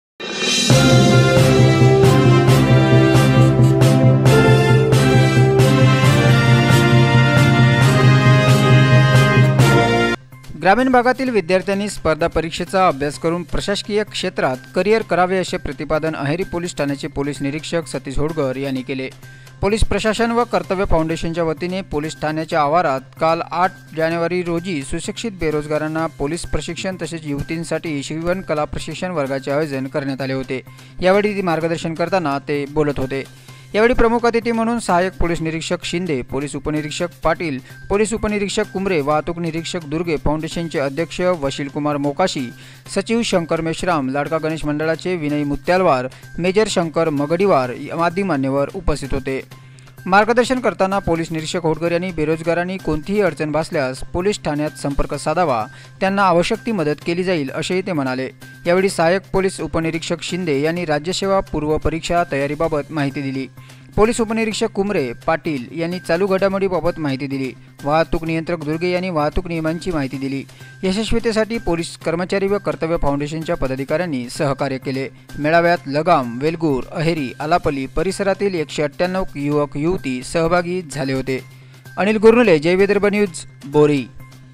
विद्याथ्र्यांनी स्पर्धा परिक्षेचा अभ्यास करावा
पोलीस निरीक्षक सतीश होडगर यांचे प्रतिपादन